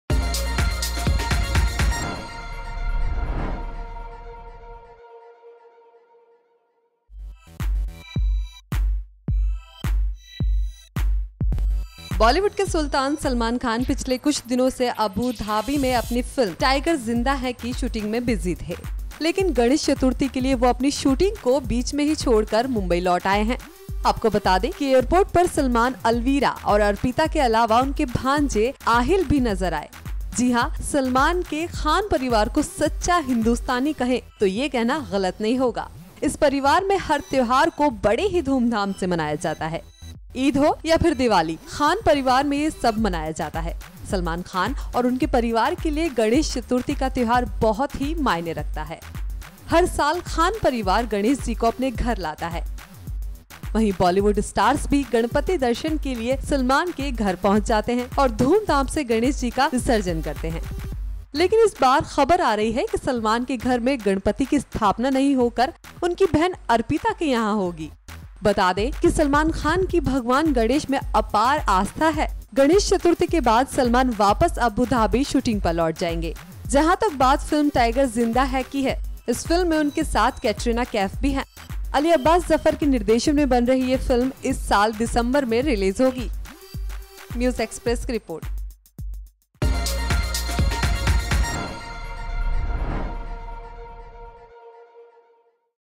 It's Ganpati time: Salman Khan takes a break from shooting
Salman Khan has taken a break from his schedule of Tiger Zinda Hai in Abu Dabi and has returned to the city for celebrating Ganesh Chaturthi. He was seen wearing a black t-shirt and denims.

Story:-Darshika
Edit:-Pardeep Tomar